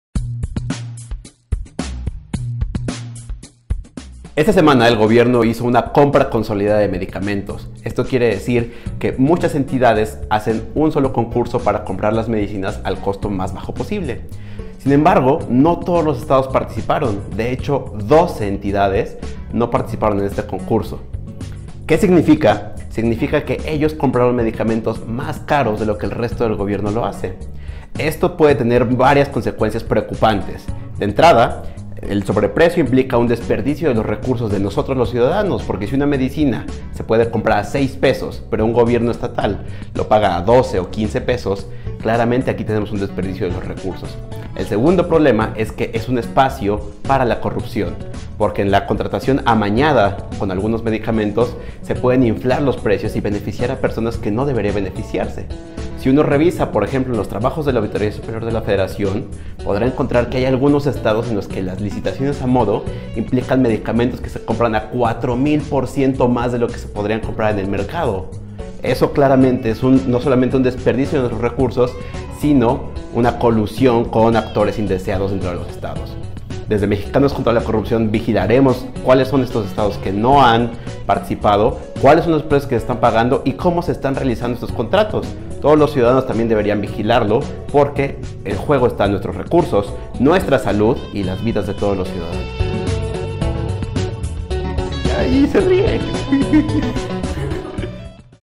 Compras consolidadas y desviación de recursos
Leonardo Núñez analiza sobre las compras consolidadas y el gasto de recursos públicos.